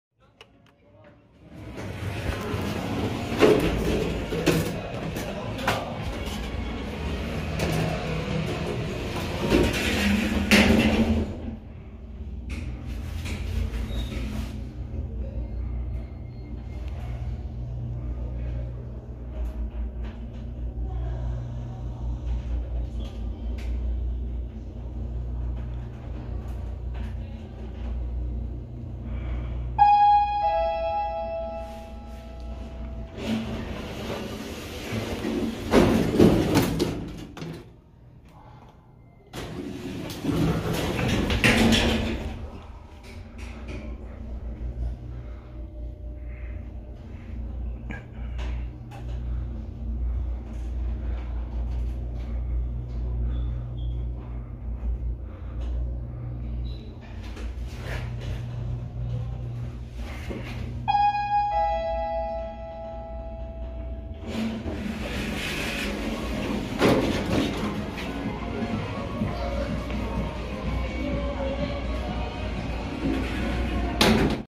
(상태 최악) 경상남도 김해시 외동 옥정프라자 LG산전엘리베이터 대차 전 탑사기
[엘리베이터 정보]
건물명: 옥정프라자
기종: LEI
승강기번호: 8012-690
설치: 1997년 10월
속력: 60
상태: Worst

[엘리베이터 특징]
현재는 2024년 9월 4일 OTIS엘리베이터 ACD5MRGM1(M)으로 대차되었습니다.

상태가 개판이다.
아마 김해에서 가장 상태가 안좋은 엘베일듯.

우선 외부 호출 버튼은 VP 시리즈 모양이다.
엘리베이터 문이 열리는 순간
문짝 상태와 내부를 보고
진심 타야하나 싶은 생각이 들었다.
내가 ㅅㅂ 엘덕한다고 이런 엘베 까지 타야되나 싶었다.

지저분한 내부와, 뜯겨져 나간 천장,
고장난 화살표가 엘리베이터 상태를 말해준다.
거기다 닫힘 버튼이 씹힌다.
진짜 멀쩡한데가 없다. 차라리 대일승강기가 낫다.

이건 뭐 방치한다고 해도 믿을 정도다.
관리소장은 지금껏 뭐했는지 궁금하다.

승차감도 거의 DP를 타는 느낌이였고
타는 내내 불안할 정도였다.
알림음은 의외로 전자차임.

작년 9월 중에는 갇힘사고도 일어났다.

아무튼 대차가 시급한 엘리베이터다.